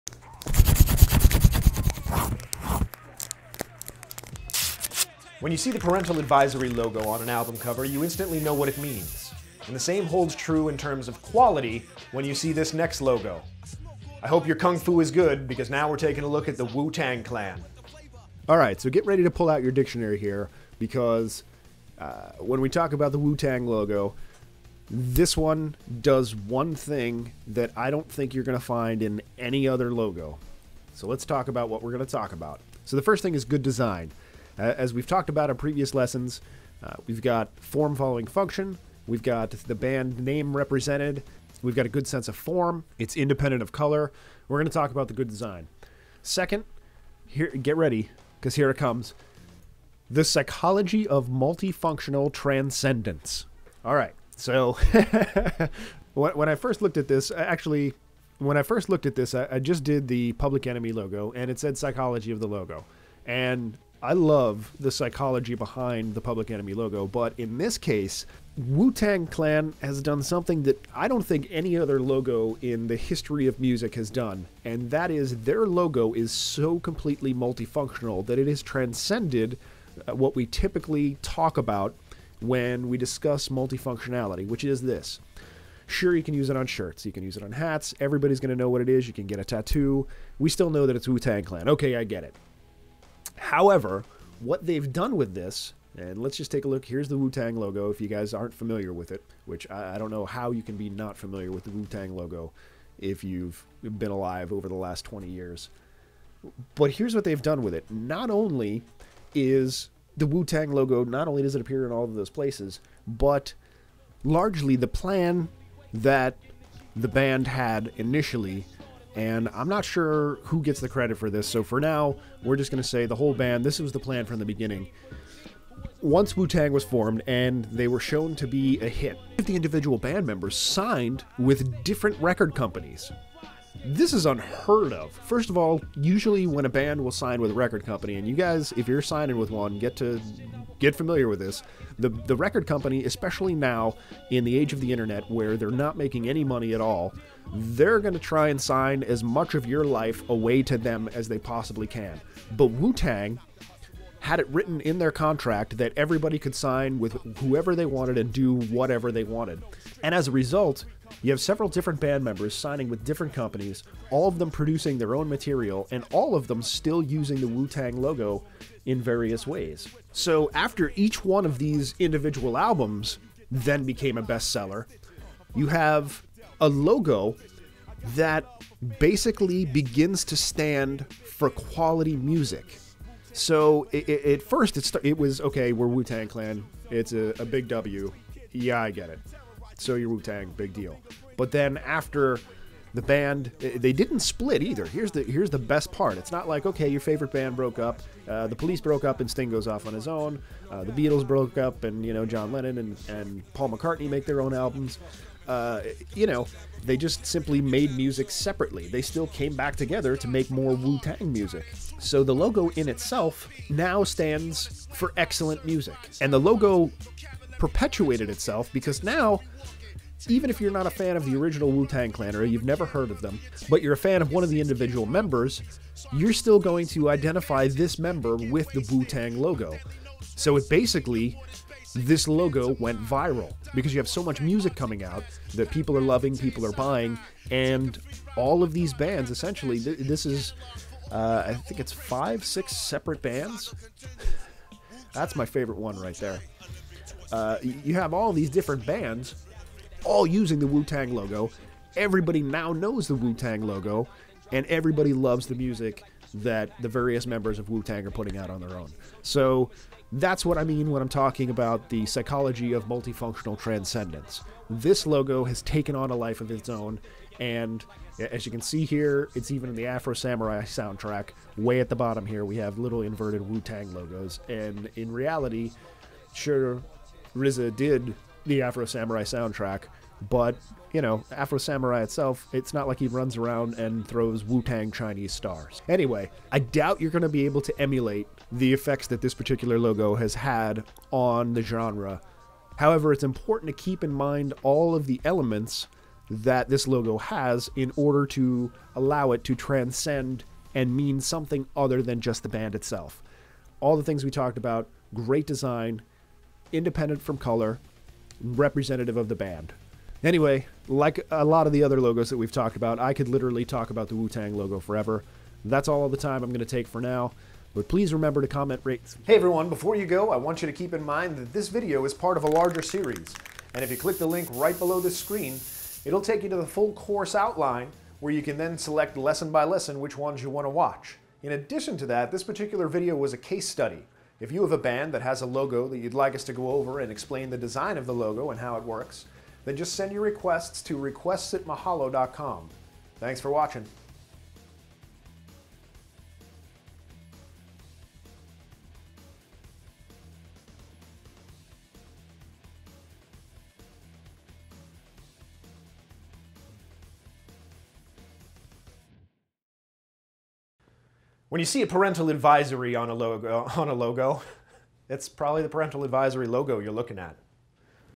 Photoshop Tutorial - Logo Case Study - Wu Tang Clan
The Wu Tang Clan logo has become more than just an icon for the group.  It has become a symbol of quality music which allows its members to pursue their own creative endeavors at different record labels and with a variety of collaborators.